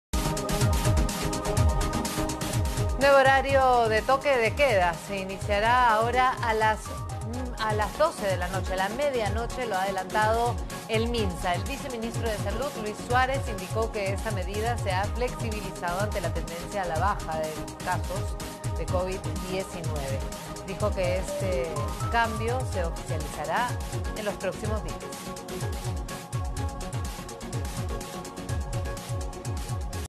El viceministro de Salud anunció cambios en horario de inmovilización, se iniciará a la medianoche
Inmovilización social: Minsa informa que Iniciará a la medianoche:

EL VICEMINISTRO DE SALUD, LUIS SUÁREZ, ANUNCIÓ CAMBIOS EN EL HORARIO DE INMOVILIZACIÓN SOCIAL, SE INICIARÁ A LA MEDIANOCHE

El viceministro de Salud, Luis Suárez Ognio, adelantó que el horario del toque de queda se reducirá y tendrá vigencia desde la medianoche hasta las 4 de la mañana. Actualmente, la medida restrictiva comienza a las 11 de la noche.

Suárez Ognio comentó que la baja incidencia del coronavirus en el Perú permite ir reduciendo las medidas más restrictivas para la población; sin embargo, enfatizó, no se debe bajar la guardia.

"La muy baja incidencia del mal que tenemos ahora nos permite ir reduciendo las medidas más estrictas, como por ejemplo, el toque de queda, que se va a cambiar de horario para iniciar a las 00.00 horas y hasta las 4 de la mañana", señaló.

"El hecho de que estemos reduciendo la incidencia no significa que ha cesado la transmisión o que ha desaparecido el riesgo de contagio. Todos los días siguen habiendo casos graves o pacientes hospitalizados por COVID-19. Si bien los casos se han reducido mucho, el riesgo sigue existiendo", agregó.

Sobre la prórroga del estado de emergencia sanitaria decretada hoy por el Ejecutivo, indicó que mientras exista transmisión de COVID-19 en el país, por más mínimo que sea, las autoridades deberán mantener el estado de emergencia sanitaria.